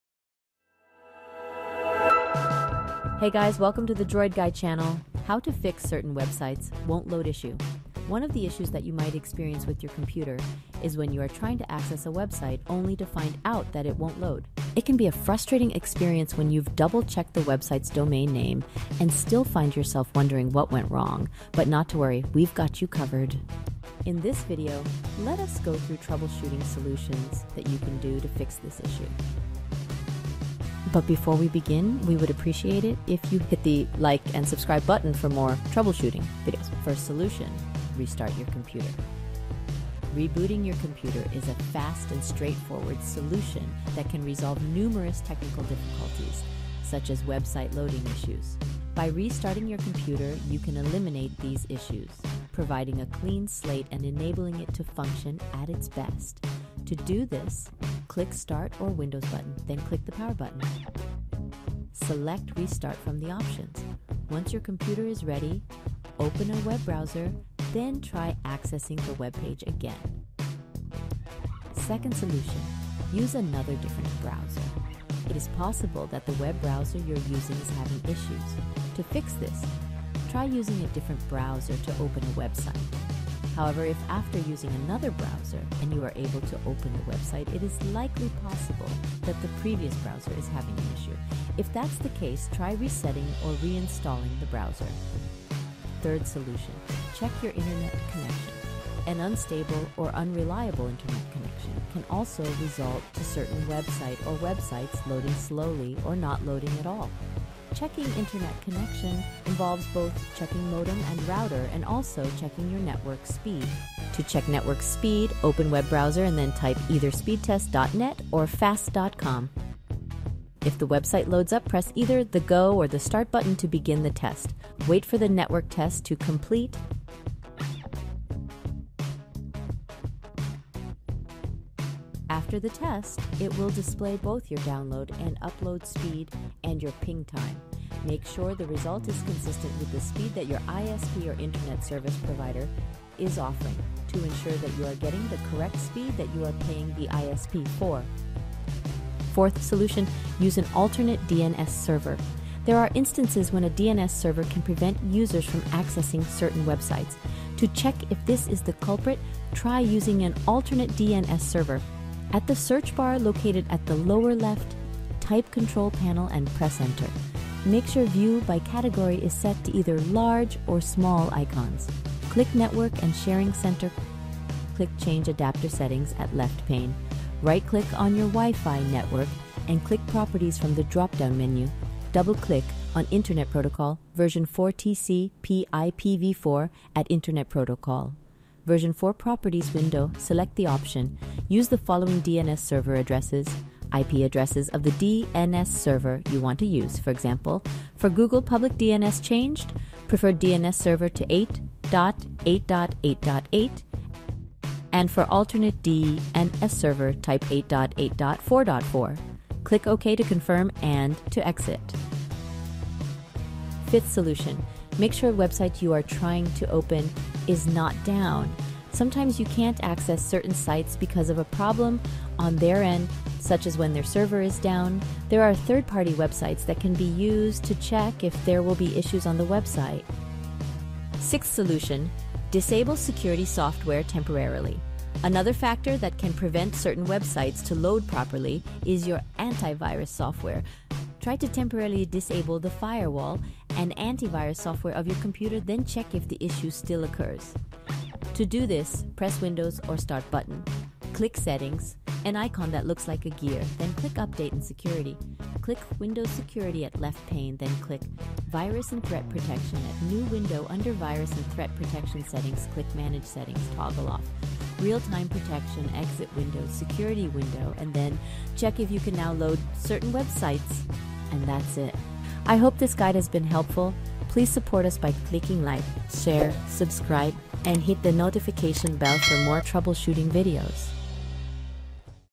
How To Fix Certain Websites Won’t Load Issue
If you cannot load certain websites on your web browser, not to worry we've got you covered. In this video, we will have gathered troubleshooting solutions that you can do to fix this issue.

This video aims to fix the issue so that you can get back to browsing again your favorite websites.

00:00 - 00:30  Introduction
00:37 - 01:12 Restart your computer
01:13 - 01:36 Use another different browser
01:37 - 02:28 Check your internet connection
02:29 - 03:33 Use an alternate DNS server
03:34 - 03:50 Make sure Website is not down
03:51 - 04:30 Disable security software temporarily